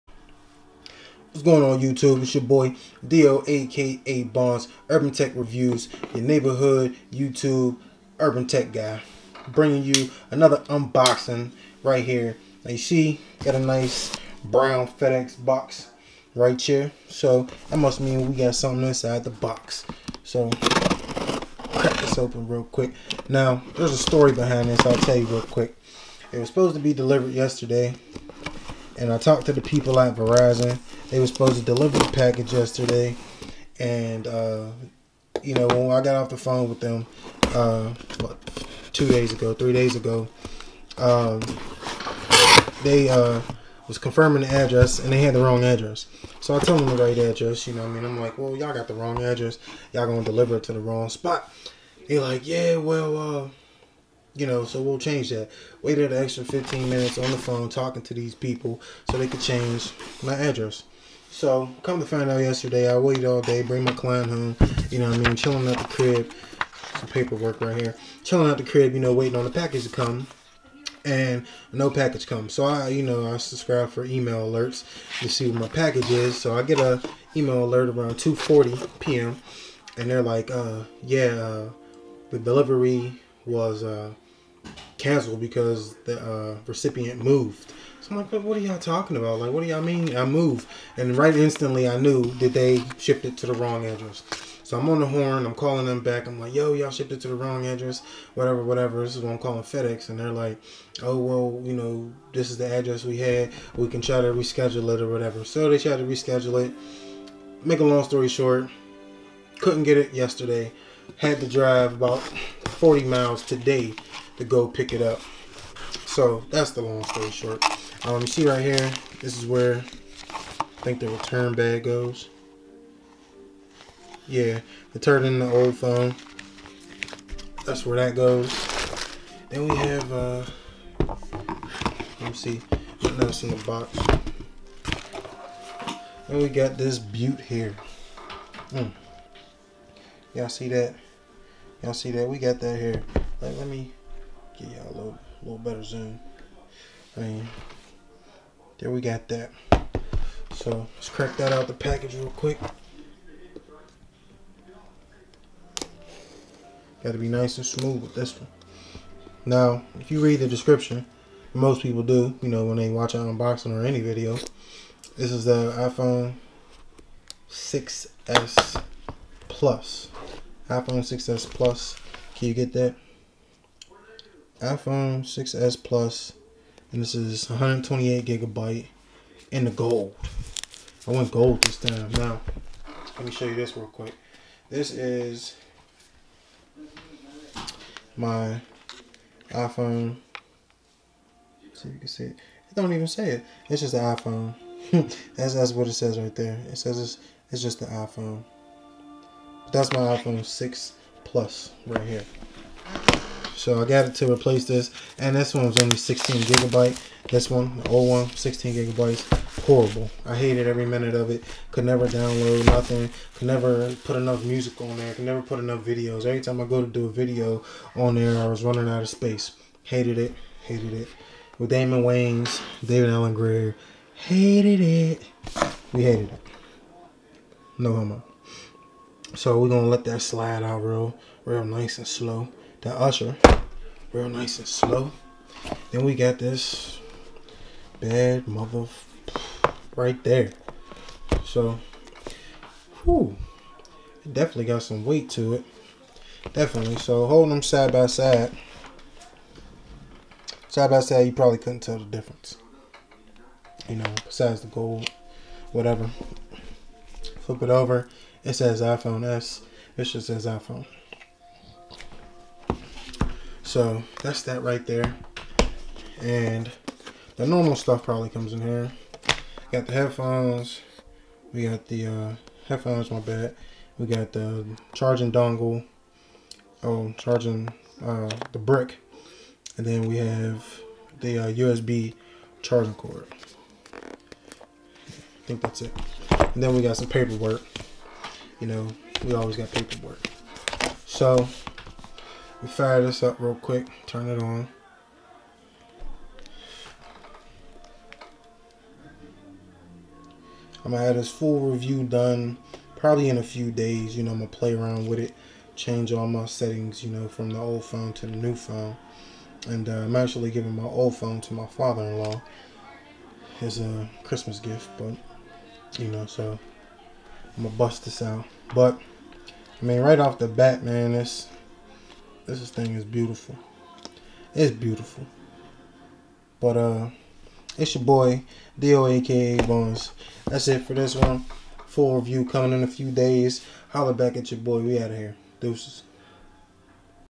APPLE IPHONE 6s PLUS UNBOXING {GOLD}
IN THIS VIDEO I WILL BE UNBOXING A BRAND NEW APPLE IPHONE 6s PLUS FROM VERIZON.